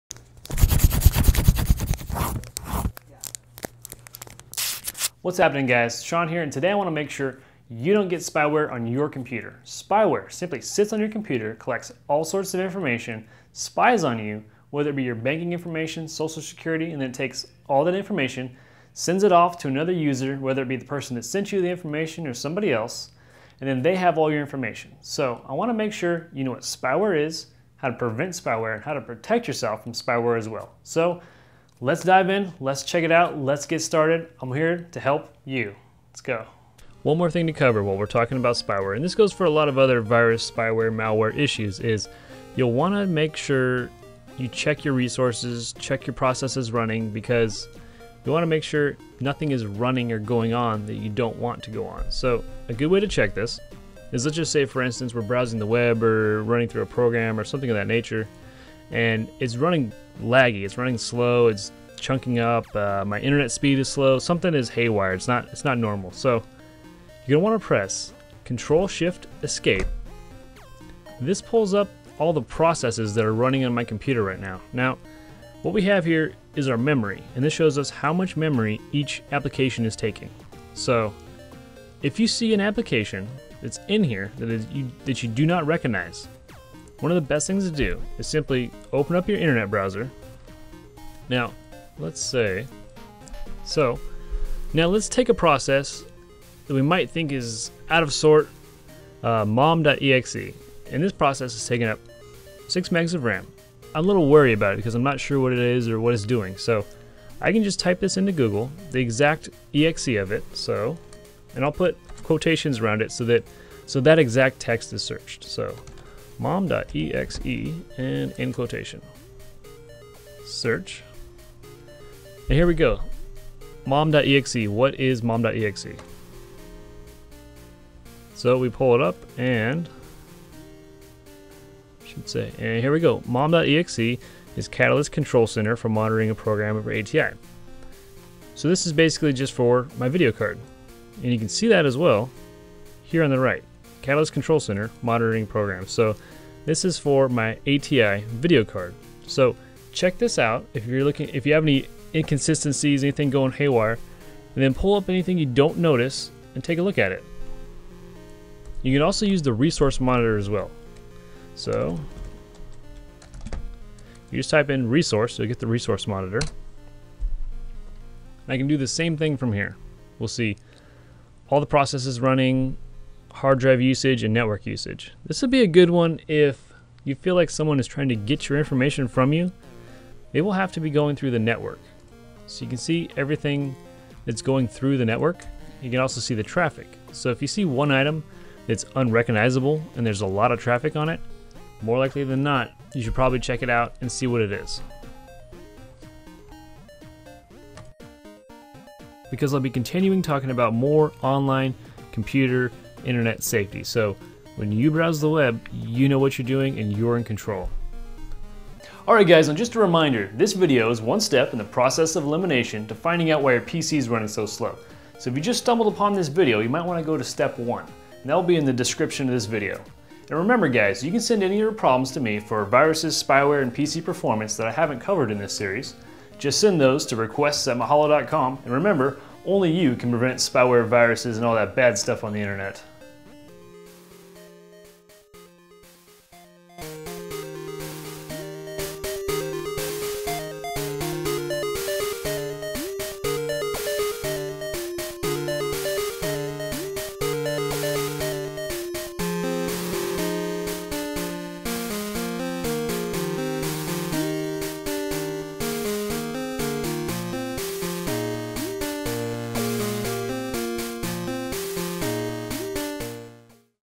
Fix Your Slow PC - Spyware Processes
In this video, Mahalo computer expert Sean Hewitt explains what you need to know about spyware processes.

What Is Spyware?

Spyware sits on your computer and collects your personal information, such as bank activity, social security number and other details your computer may contain. The spyware then sends this information to another user, which may be the person who put it there or someone else altogether. This process can cause your computer to slow down.

If you're browsing the web and your computer is lagging or your internet speed is slow, you can check your computer's processes to see if you've picked up spyware. Option 1: Process MonitorPress Ctrl+Shift+Esc. This will show you all the processes running on your computer and how much memory each is using up.Examine the processes and look for any applications you don't recognize. If you do find a name that seems foreign to your computer, use an internet search engine to find out what it is. Make sure you search for the exact ".exe" name of the application. Use this research to determine if the application is spyware or not. Option 2: Resource MonitorSearch for "resource" on your computer and open the resource monitor. This monitor will contain the same information as the process monitor but also shows the hard drive and network usage. If you think someone is trying access your information through your network, this tool will show you all the processes using the network. Again, if you find any unrecognizable items with a high level of traffic, you should research the name to determine if it is spyware.

This is only one section in a series of steps you can take to fix your slow PC. If your computer is still running slow, you can try some of the other suggested steps.